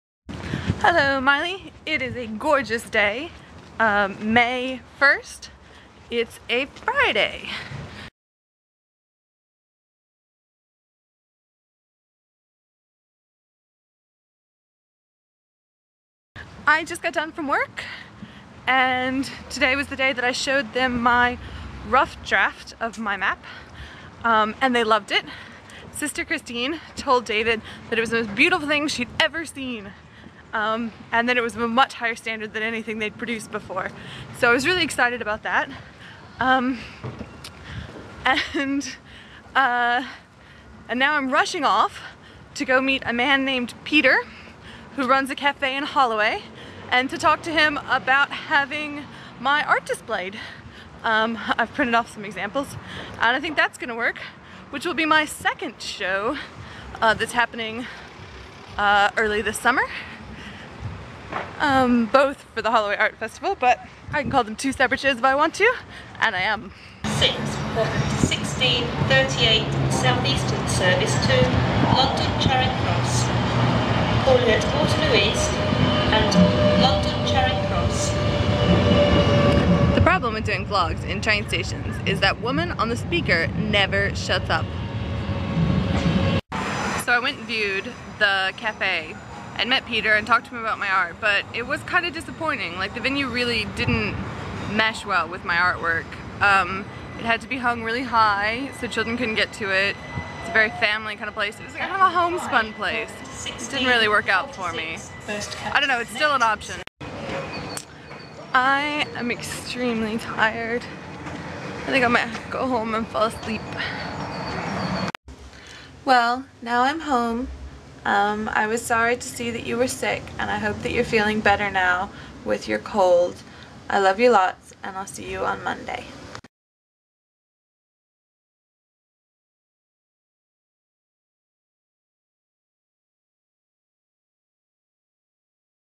may 1
in which jes talks about being liked at work, seeing an art venue, and then being tired. also, a car that got flipped over.